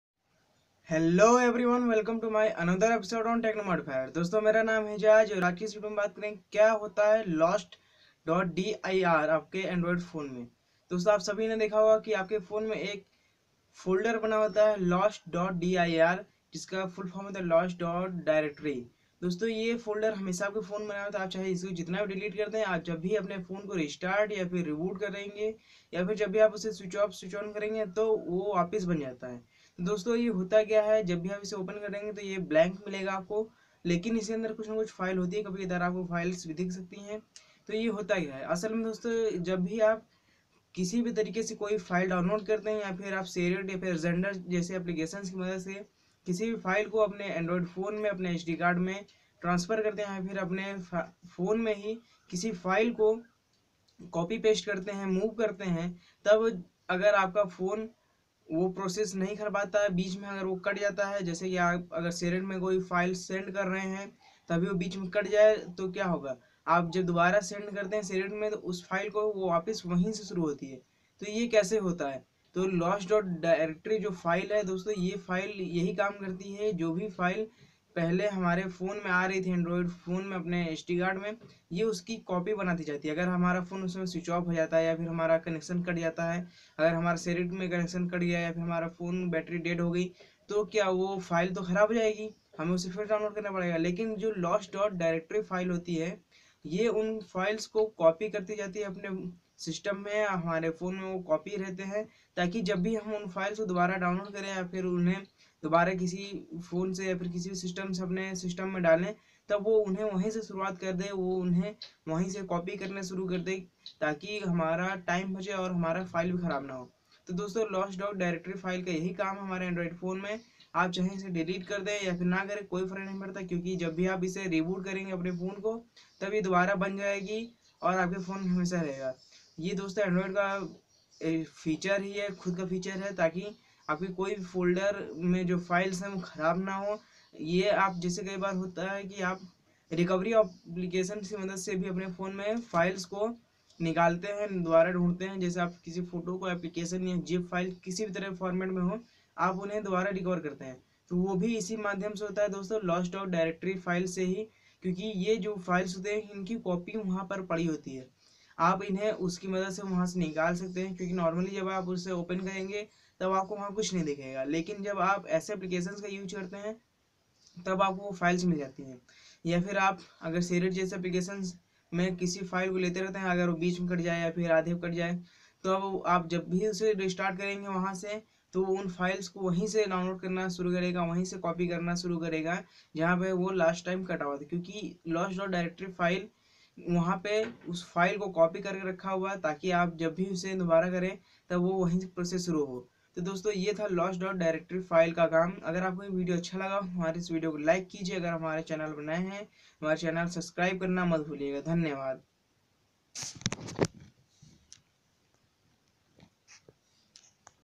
What is lost.dir on Android phone
Hello everyone in this video I'm showing you what is lost.dir on Android phone.